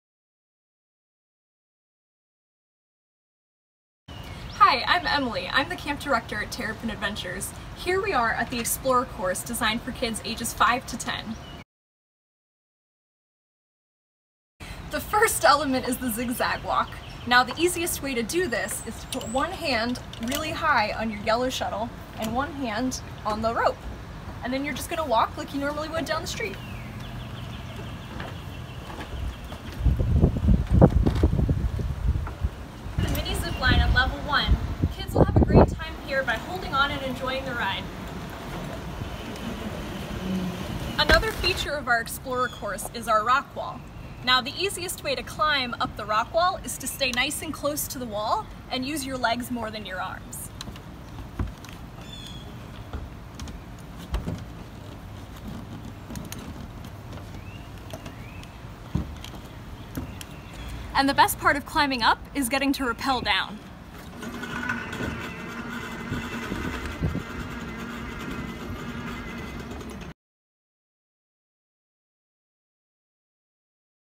Tour of the Course: Terrapin Explorer Kids Course at Terrapin Adventures
Enjoy this first-hand look at some of the coolest elements at our Adventure Park!

The Terrapin Explorer Kids Course offers a mental and physical challenge for your kids -- ages 5-10 years old. Two levels of tantalizing traverses feature more than 20 obstacles to overcome! If that's not enough, there's also a 20-foot Rock Climbing Wall to tackle!
In this installment, we teach you tips for how to navigate some of our favorite elements, including the Mini-Zip.
Book your next Kids Course adventure at: www/